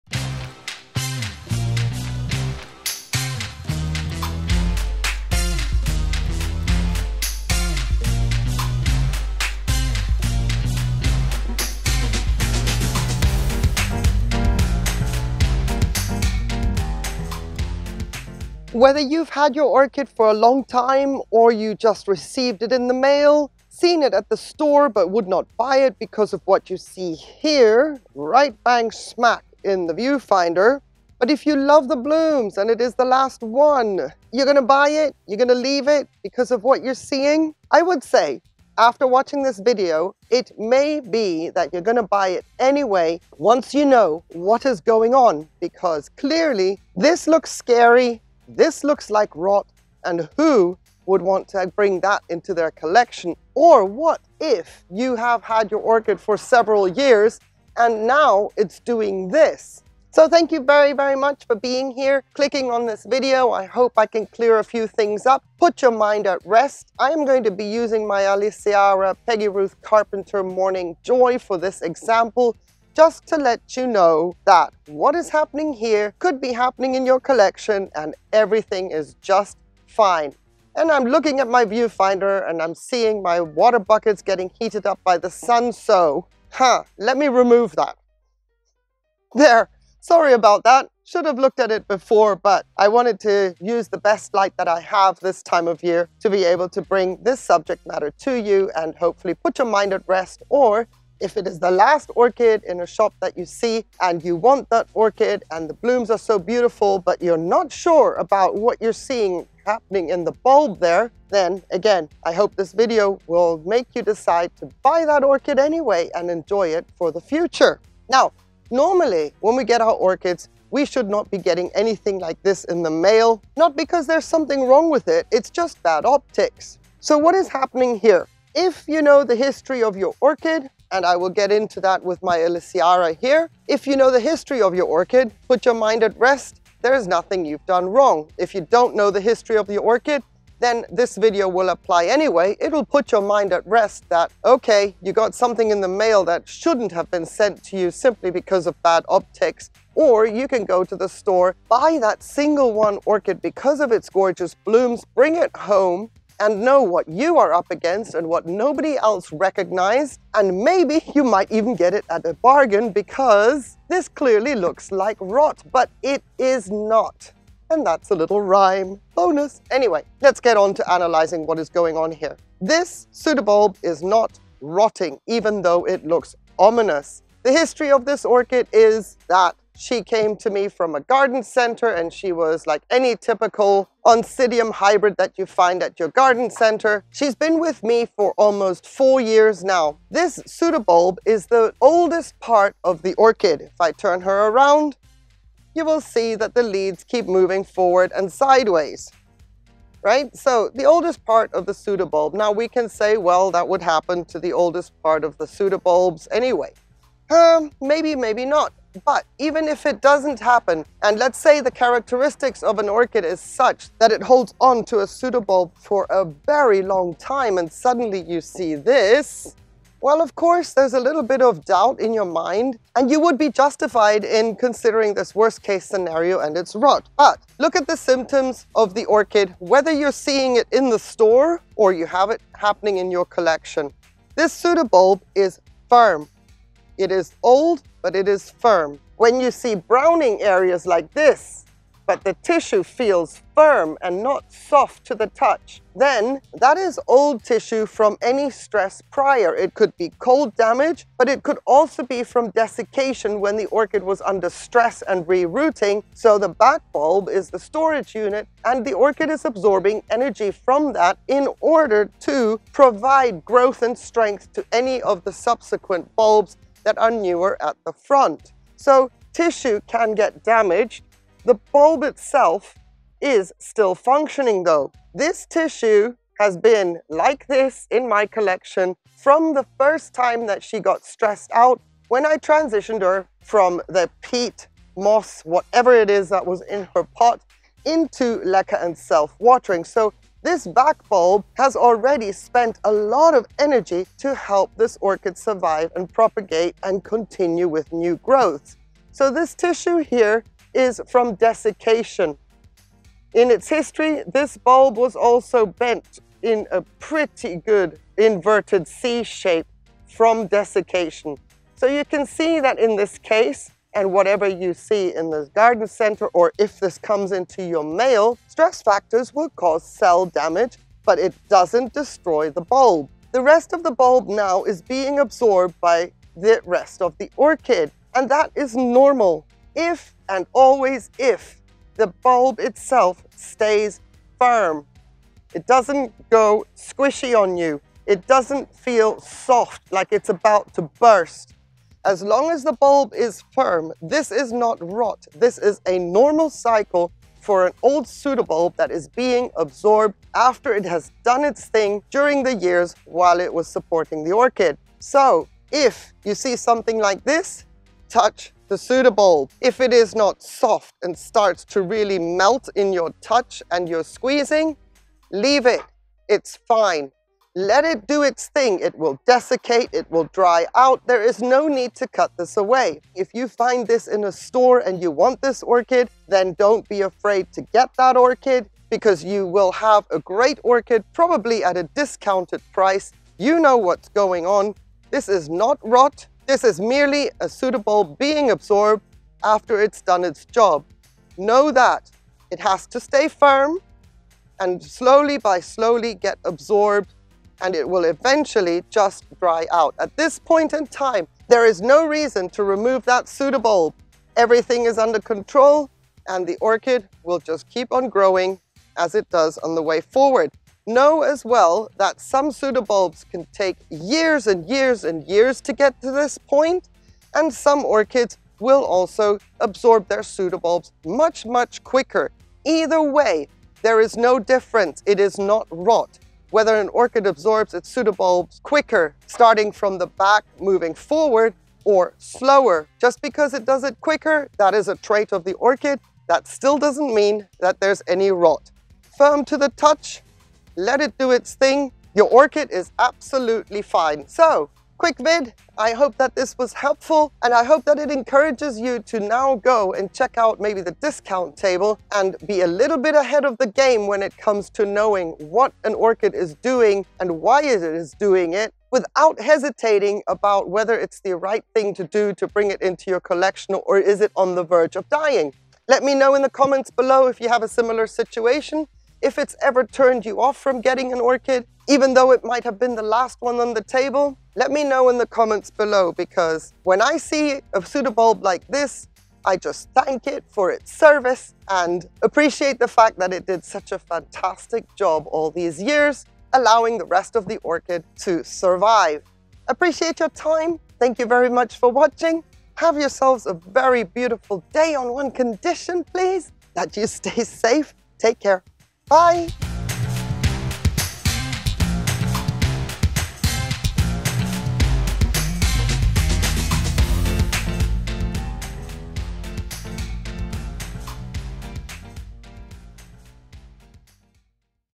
KNOW your ROT OR NOT | Buy that discounted orchid with confidence 👍🏼
Knowing the symptoms of rot or desiccation will make your bargain hunter a lot more fun and rewarding! 🤩Orchid Care for Beginners or, anyone who is not quite sure about what they are seeing 😃
My environment:
Mediterranean climate, hot summers / mild winters
45% - 85% humidity, with regular airflow
Orchid growing supplies:
               Seramis, Akadama
Water: Reverse Osmosis
Insecticidal Soap
General PPM: 300 Summer, or in active growth
                          150 Winter, RO Water and KelpMax only